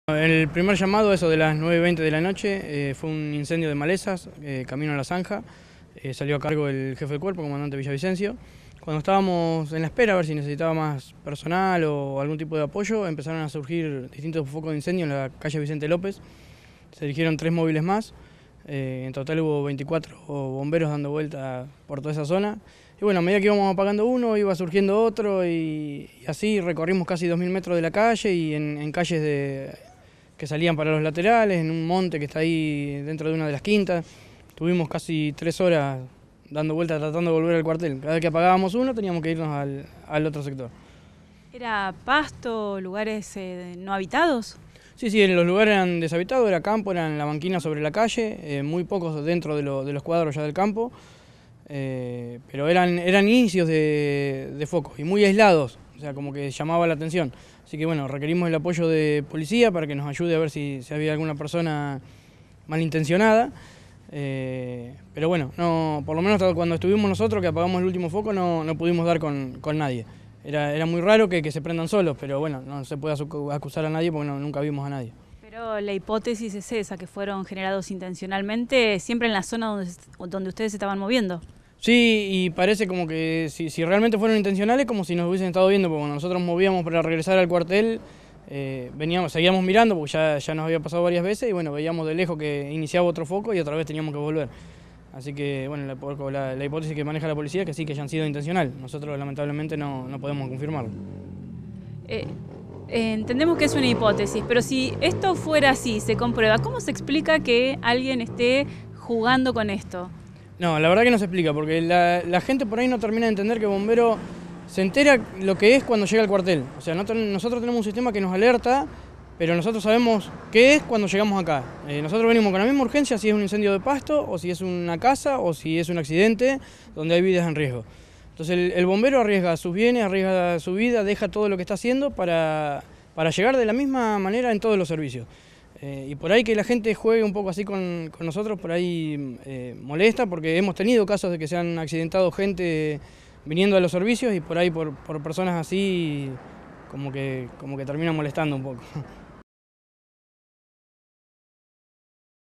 Cuatro incendios sospechosos en la misma noche
Los focos fueron combatidos por 24 bomberos durante 3 horas, en la zona rural y en la prolongación de la calle Vicente López y las xonas aledañas.